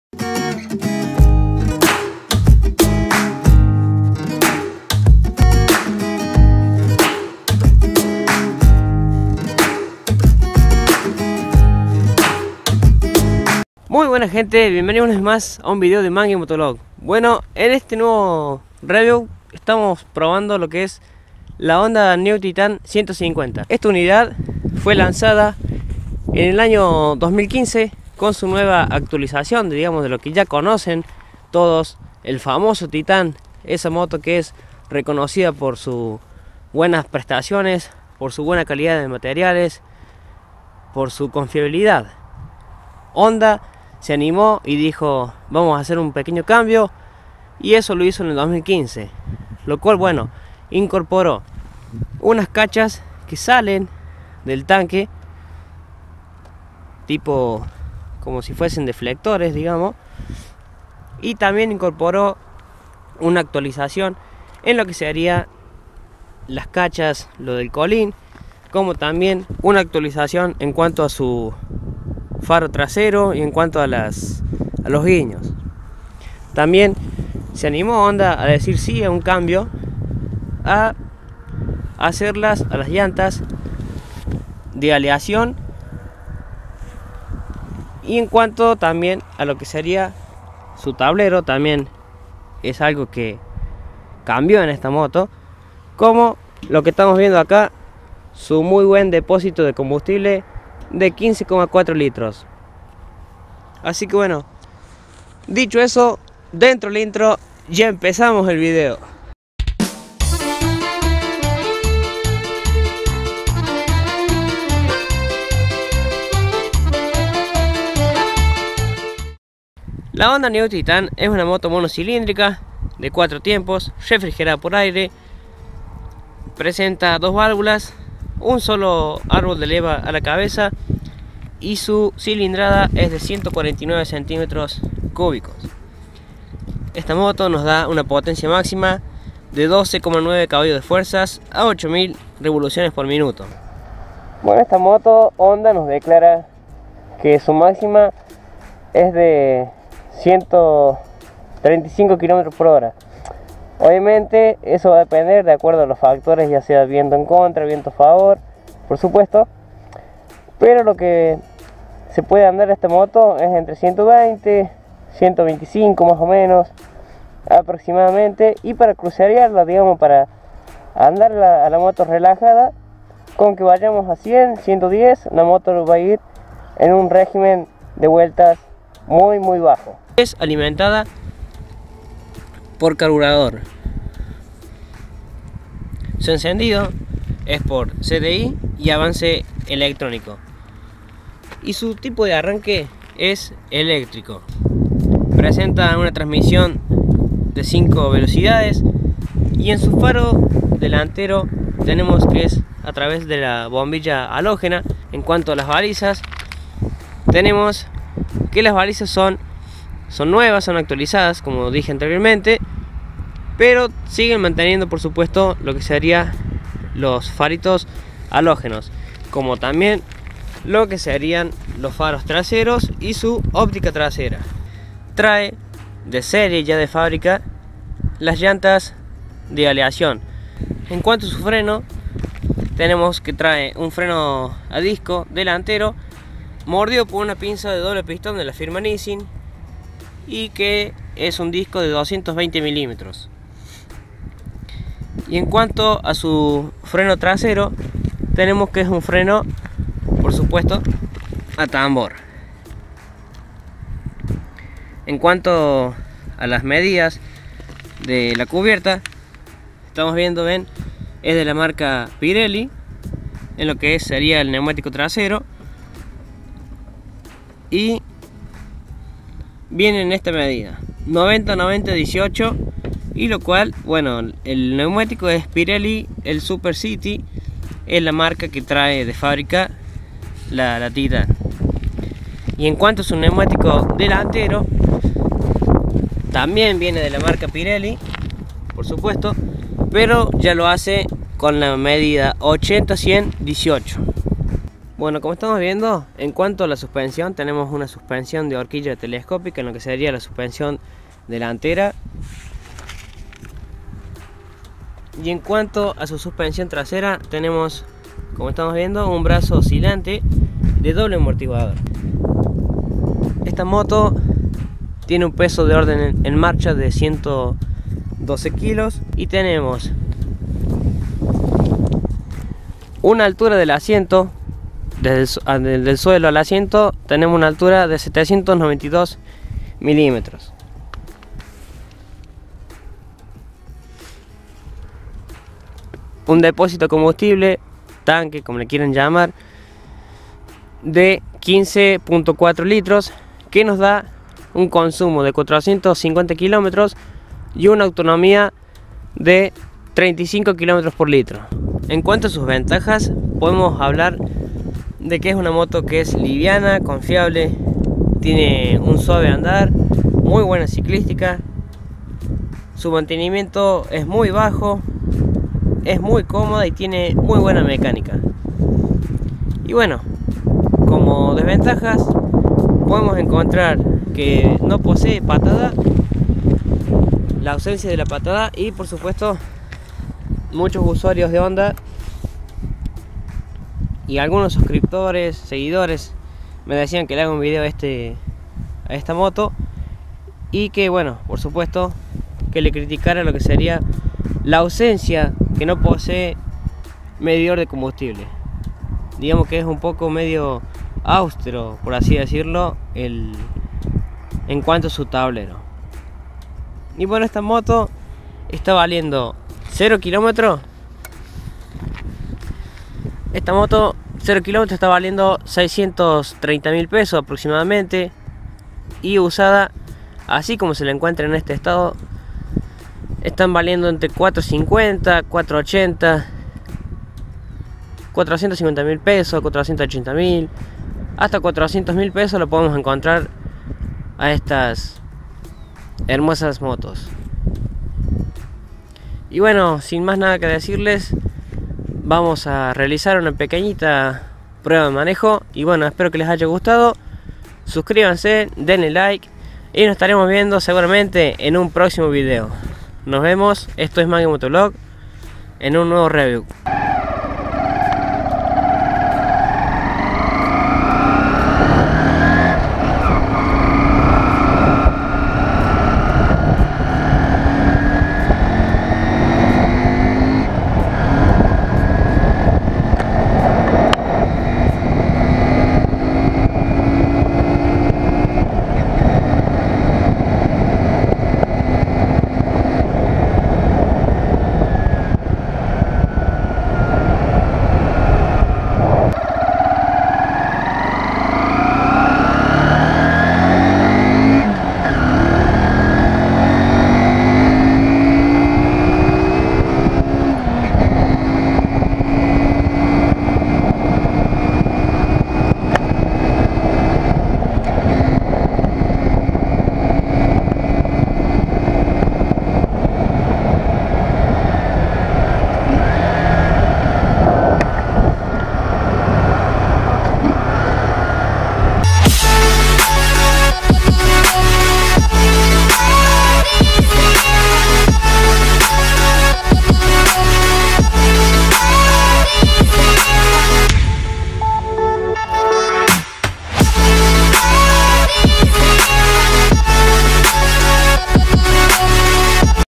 REVIEW HONDA CG NEW TITAN 150 | HONDA CG NEW TITAN 150 CARACTERÍSTICAS, LO BUENO Y MALO DE LA MOTO
MI EQUIPAMIENTO:
MOTO: YAMAHA FZ16